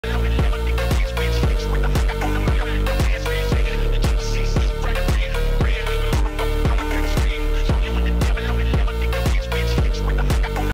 German Shepherd Gets Pranked 😳 Watch Her Reaction !
German Shepherd Gets Pranked By Dog Mask!